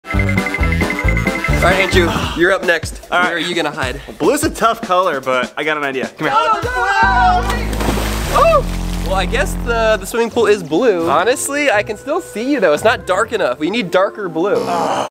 WHAT IS THE BEST HIDING SPOT FOR BLUE???
Today Lizzy and Team Rar are competing in One Color Hide and Seek!! Andrew is the color blue and he has to find his hiding spot!!